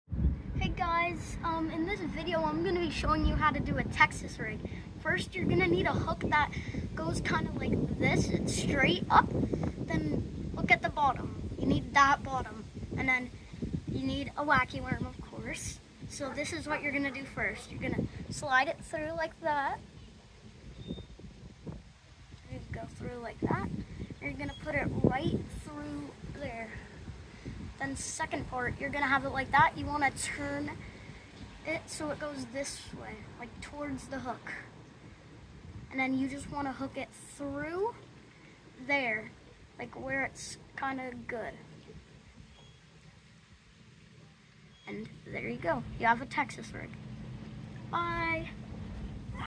Texas Rig Demo for a Wacky Worm
This is how you do a Texas Rig with your bait!